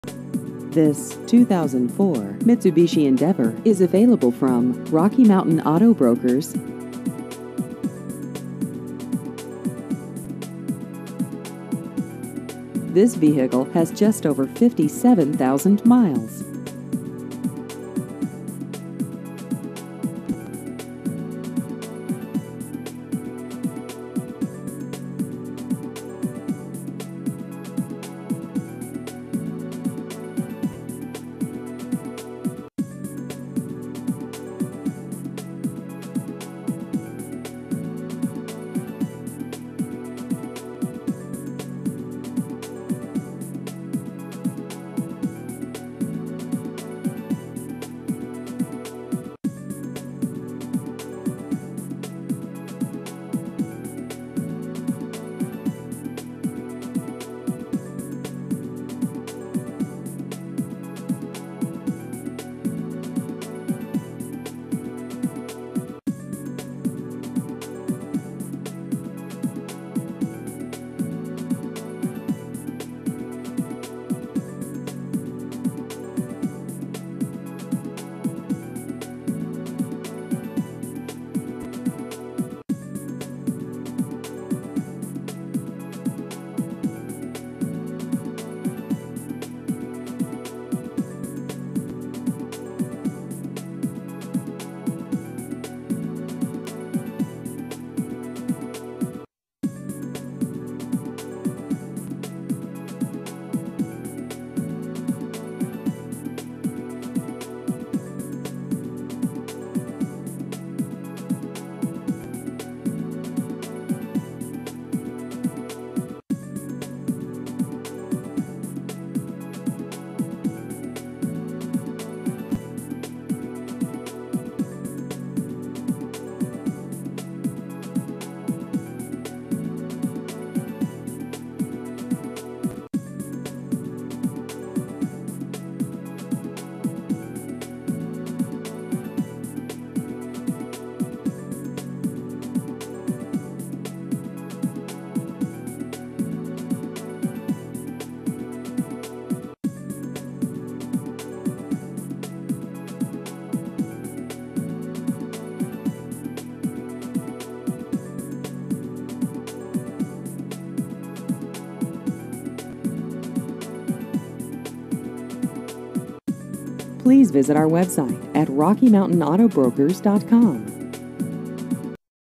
2004 Mitsubishi Endeavor Used Cars Colorado Springs CO
This 2004 Mitsubishi Endeavor is available from Rocky Mountain Auto Brokers.